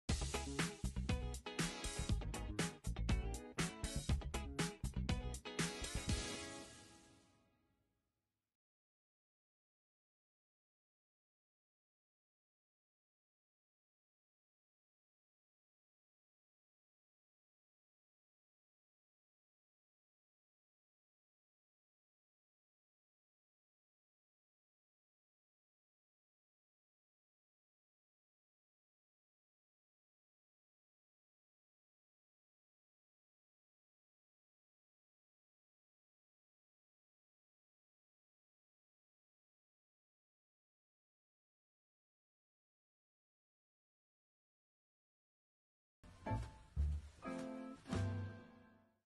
Banderas Rojas en el amor
Domingo 21.4.24 En vivo con una amiga suscriptora
Nos hablará de una historia terrible con un  chico que lo ayudó a salir de su país lo vistió y mantuvo sin saber que era tremendo Paquete completo
12 horas de Perú 🇵🇪 y 19 de Alemania 🇩🇪